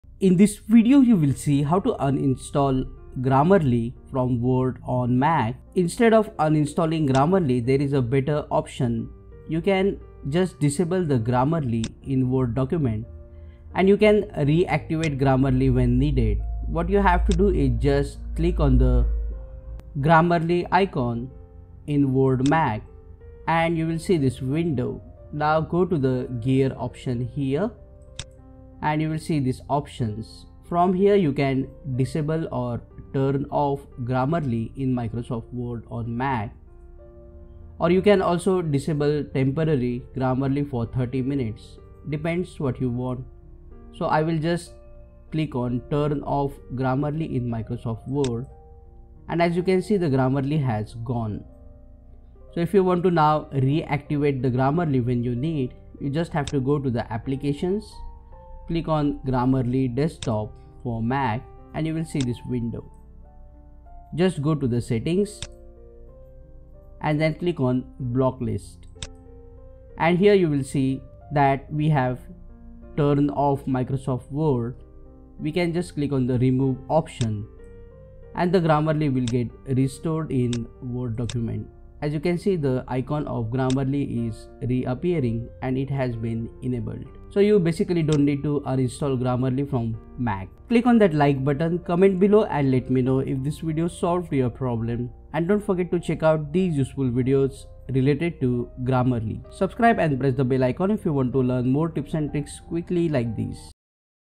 How To Uninstall Grammarly From Word MAC
This video shows How To Uninstall Grammarly From Word MAC on MacBook Air or Pro [ ✅✅✅ GRAMMARLY ] but instead of uninstalling Grammarly their is a better option you can simply turn off or disable Grammarly in WORD MAC/ the best thing is that you can Enable Grammarly on Word MAC then you need it
0:00 Into
0:16 Turn Off Grammarly in Word MAC
0:51 Turn ON / Enable Grammarly in Word MAC